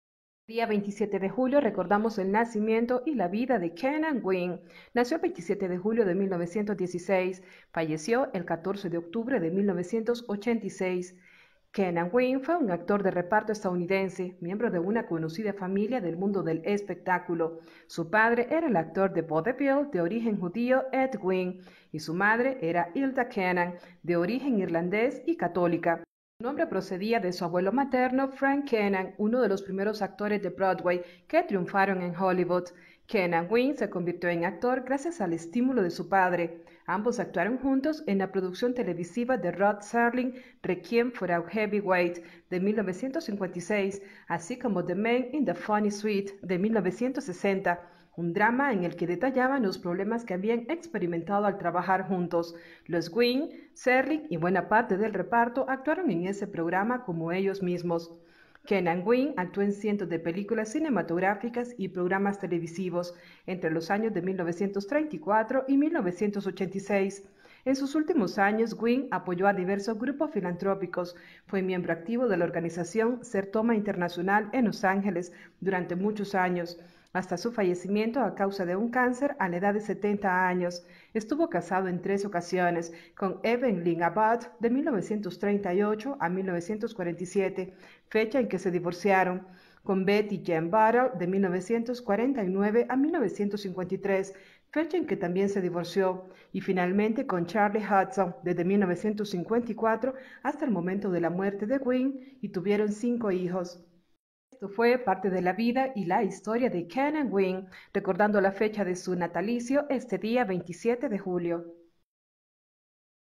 KEENAN WYNN
Nacimiento 27 de julio de 1916
Fallecimiento 14 de octubre de 1986 (70 años)

Biografía: fue un actor de reparto estadounidense, miembro de una conocida familia del mundo del espectáculo. Su padre era el actor de vodevil de origen judío Ed Wynn, y su madre era Hilda Keenan, de origen irlandés y católica. Su nombre procedía de su abuelo materno, Frank Keenan, uno de los primeros actores de Broadway que triunfaron en Hollywood. Keenan Wynn se convirtió en actor gracias al estímulo de su padre. Ambos actuaron juntos en la producción televisiva de Rod Serling Requiem for a Heavyweight (1956), así como en The Man in the Funny Suit (1960), un drama en el que detallaban los problemas que habían experimentado al trabajar juntos; los Wynns, Serling, y buena parte del reparto actuaron en ese programa como ellos mismos. Keenan Wynn actuó en cientos de películas cinematográficas y programas televisivos entre 1934 y 1986.

En sus últimos años, Wynn apoyó a diversos grupos filantrópicos. Fue miembro activo de la organización Sertoma International, en Los Ángeles, durante muchos años, hasta su fallecimiento a causa de un cáncer a los 70 años de edad. Estuvo casado en tres ocasiones: con Eve Lynn Abbott de 1938 a 1947, fecha en que se divorciaron; con Betty Jane Butler, de 1949 a 1953, fecha en que también se divorció, y finalmente con Sharley Hudson, desde 1954 hasta el momento de la muerte de Wynn. Tuvo cinco hijos.

Wikipedia in English Biography:

Died October 14, 1986 (age 70)

He was an American supporting actor, member of a well-known show business family. His father was Jewish-born vaudeville actor Ed Wynn, and his mother was Hilda Keenan, of Irish and Catholic origin. His name came from his maternal grandfather, Frank Keenan, one of the first Broadway actors to make it big in Hollywood. Keenan Wynn became an actor thanks to the encouragement of his father. The two acted together in the television production of Rod Serling Requiem for a Heavyweight (1956), as well as in The Man in the Funny Suit (1960), a drama in which they detailed the problems they have experienced working together; the Wynns, Serling, and much of the cast performed on that show as themselves.

Keenan Wynn acted in hundreds of motion pictures and television shows between 1934 and 1986. In his later years, Wynn supported various philanthropic groups. He was an active member of the Sertoma International organization, in Los Angeles, for many years, until his death from cancer at the age of 70. He was married three times: to Eve Lynn Abbott from 1938 to 1947, when they divorced; with Betty Jane Butler, from 1949 to 1953, when she also divorced, and finally with Sharley Hudson, from 1954 until the time of Wynn's death. He had five children.

Filmography
• 1942 Somewhere I'll Find You • 1942 The War Against Mrs. Hadley • 1942 For Me and My Gal • 1942 Northwest Rangers • 1943 Lost Angel • 1944 See Here, Private Hargrove • 1944 Since You Went Away • 1944 Marriage Is a Private Affair • 1945 Without Love • 1945 The Clock • 1945 Between Two Women • 1945 Ziegfeld Follies • 1945 Week-End at the Waldorf • 1945 What Next, Corporal Hargrove? • 1946 Easy to Wed • 1946 The Thrill of Brazil • 1946 No Leave, No Love • 1946 The Cockeyed Miracle • 1947 The Hucksters • 1947 Song of the Thin Man • 1948 B.F.'s Daughter • 1948 The Three Musketeers • 1948 My Dear Secretary • 1949 Neptune's Daughter • 1949 That Midnight Kiss • 1950 Annie Get Your Gun • 1950 Love That Brute • 1952 Desperate Search • 1971 The Manipulator • 1972 Panhandle 38 • 1974 The Internecine Project ( • 1974 The Legend of Earl Durand • 1974 Hit Lady • 1974 Popcorn • 1975 Target Risk • 1975 He Is My Brother • 1975 Nashville • 1977 Mission to Glory: A True Story • 1977 Orca • 1977 Sex and the Married Woman • 1978 Laserblast • 1978 Coach • 1979 The Glove • 1979 The Treasure Seekers • 1979 A Touch of the Sun • 1980 Just Tell Me What You Want • 1980 Mom, the Wolfman and Me • 1981 The Monkey Mission • 1982 A Piano for Mrs. Cimino • 1985 Zoo Ship • 1986 Black Moon Rising • 1986 Hyper Sapien: People from Another Star

Créditos: Tomados de Wikipedia